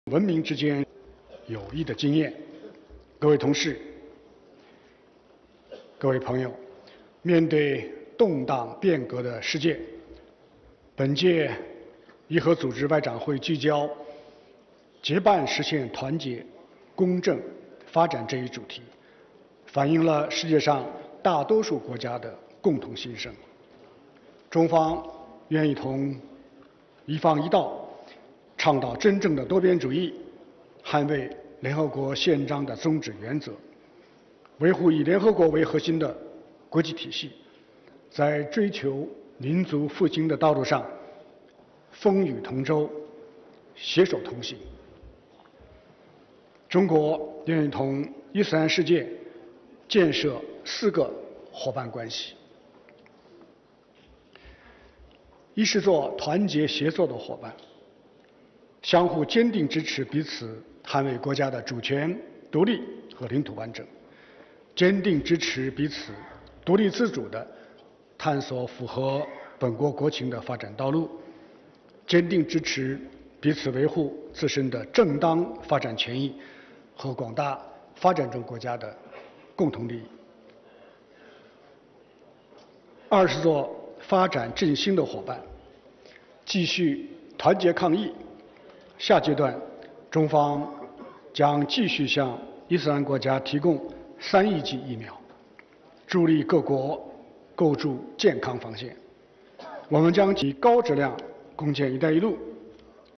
48th Session of the OIC Council of Foreign Ministers (CFM) in Islamabad, Pakistan
48th Session of the OIC Council of Foreign Ministers (CFM) kicks off today, March 22nd, 2022 in Islamabad, Pakistan, with the theme “Partnering for Unity, Justice and Development”.